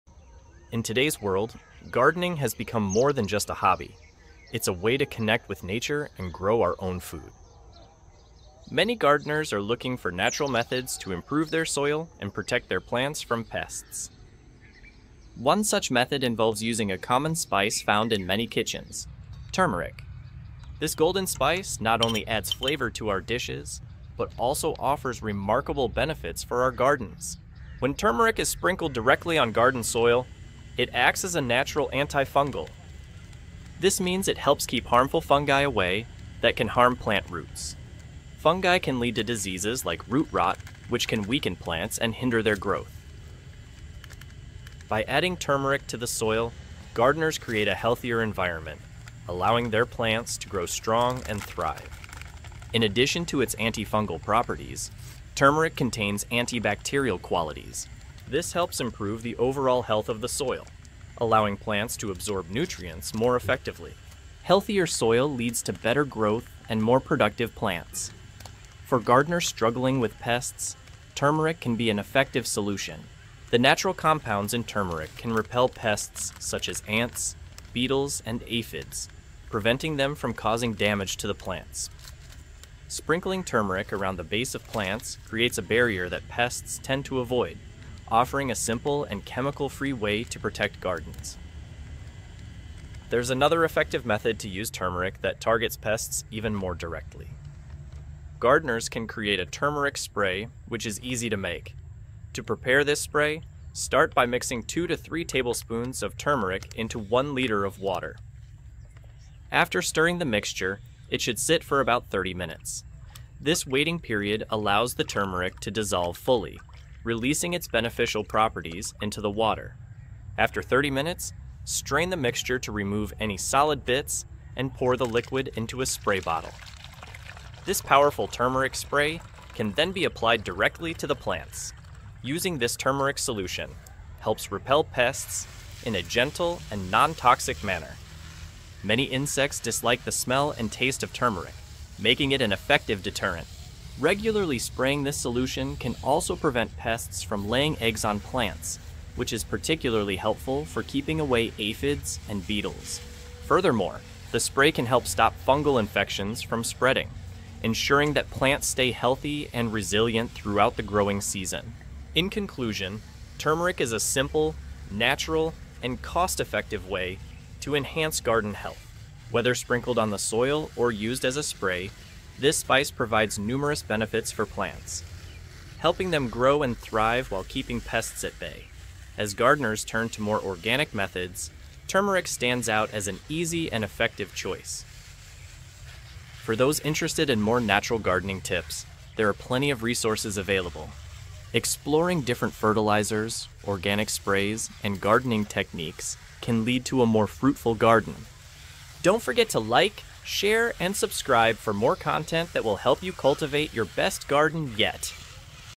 This golden powder can kill pests and make your plants explode with growth
In today’s world, gardening has become more than just a hobby; it’s a meaningful way to connect with nature and grow our own food. Many gardeners are actively seeking natural methods to improve their soil quality and protect their plants from pesky insects. One effective method involves using a common kitchen spice: turmeric. This golden spice not only enhances the flavor of our dishes but also offers impressive benefits for our gardens.

When gardeners sprinkle turmeric directly onto the soil, it acts as a natural antifungal agent. This means it helps keep harmful fungi at bay, which can damage plant roots. Fungi can lead to serious diseases like root rot, weakening plants and stunting their growth. By incorporating turmeric into the soil, gardeners can create a healthier environment that allows their plants to grow strong and flourish.